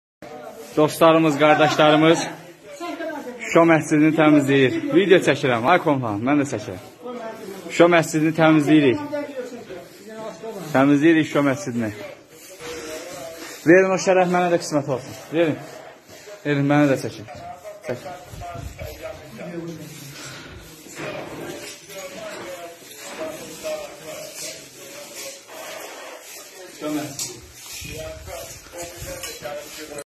Hərbçilərimiz Gövhər Ağa məscidini təmizlədilər
Azərbaycan ordusunun əsgərləri Şuşada Gövhər Ağa məscidini təmizləyib.

Bu barədə video “Telegram” sosial şəbəkəsində yayılıb.

Bir qrup hərbçimiz məscidin içindəki zibilləri süpürüblər.

Həmin görüntüləri təqdim edirik: